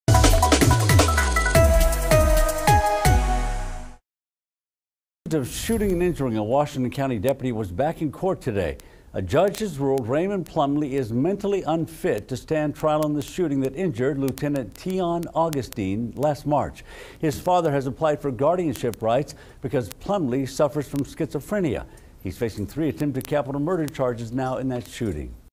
Man accused of shooting Washington County deputy appears in court
The man accused of shooting Lt. Tion Augustine was in court for a special hearing.